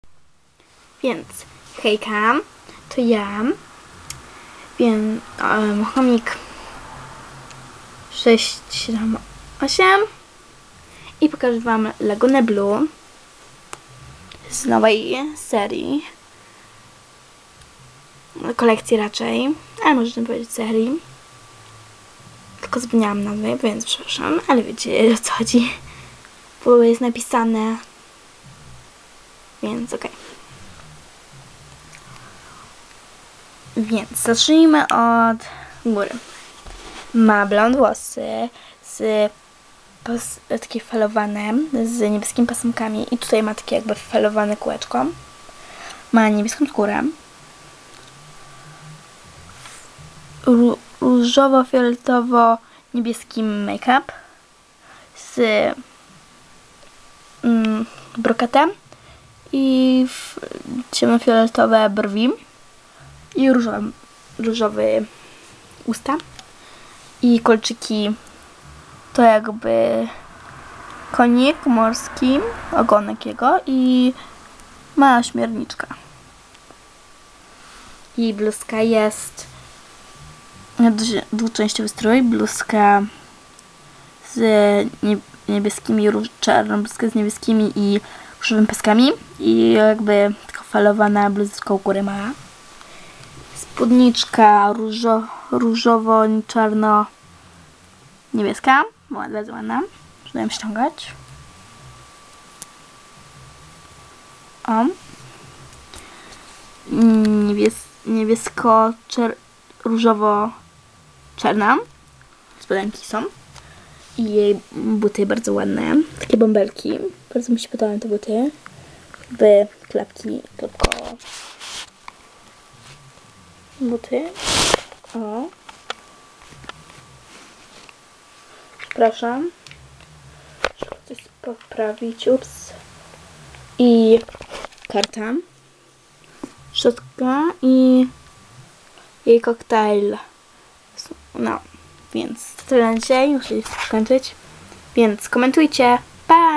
monster high skull shores lagoona blue (po polsku)
moja nowa lalka monster high skull shores jest extra! warto obejrzeć ten film po polsku!